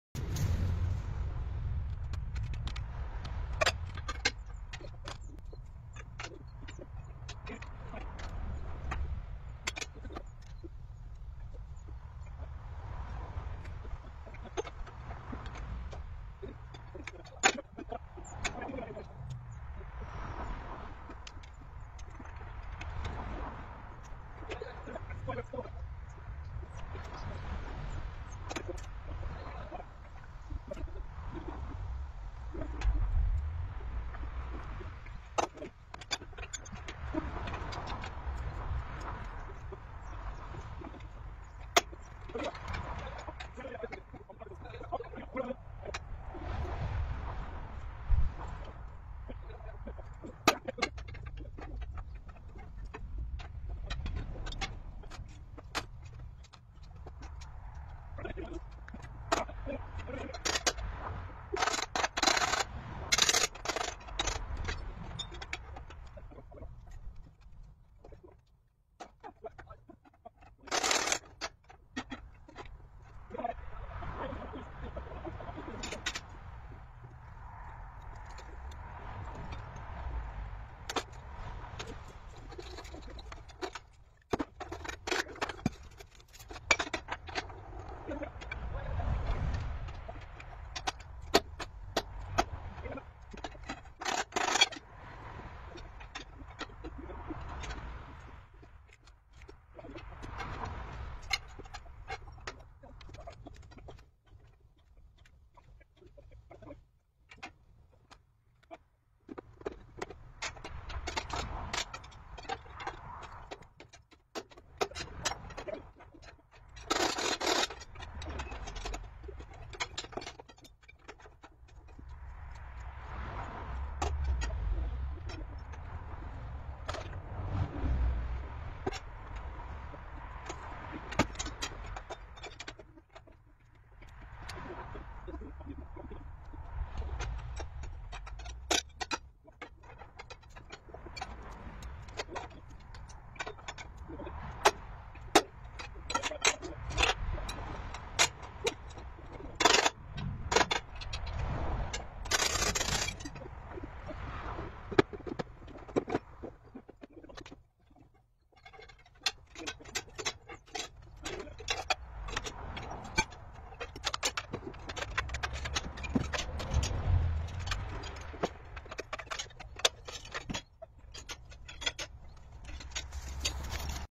Resning av SM4WWG mast/torn, del 2 av 3
Video från när vi lyfte masten/tornet på plats